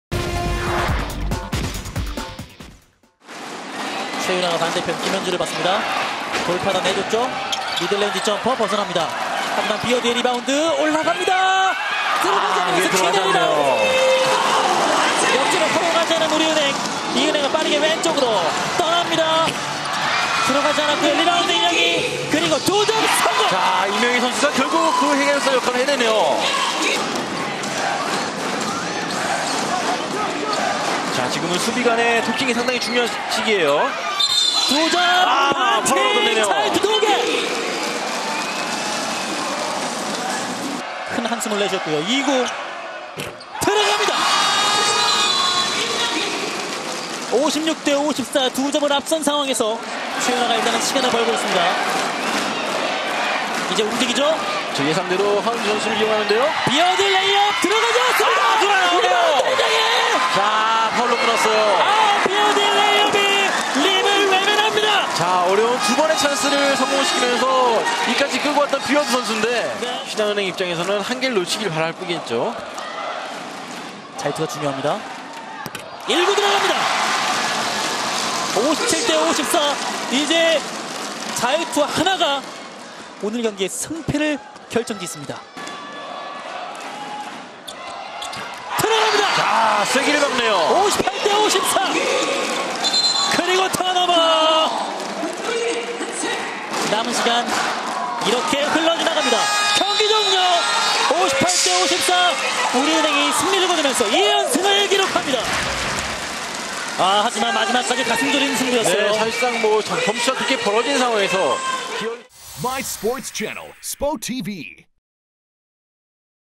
[여자농구] 챔프 2차 : 승부처에서 빛난 임영희의 존재감! (03.26)
챔프 2차 : 승부처에서 빛난 임영희의 존재감!